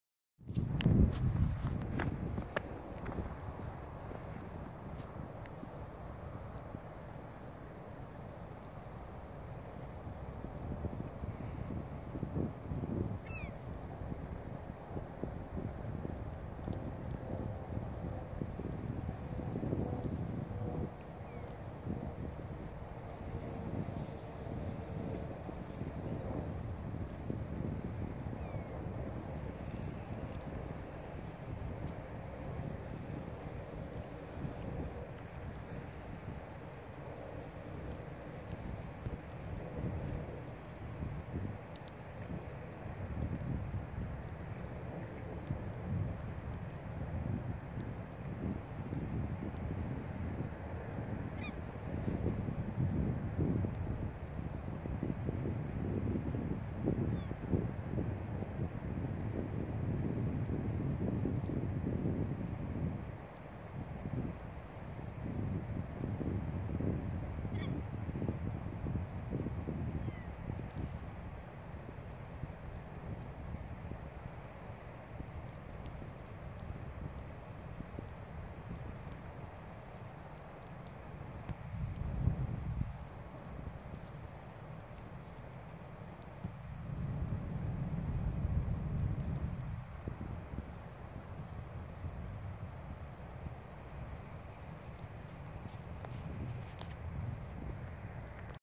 Beautiful Detroit River Ice.1 21 2015
This ice looked like smoke in the water.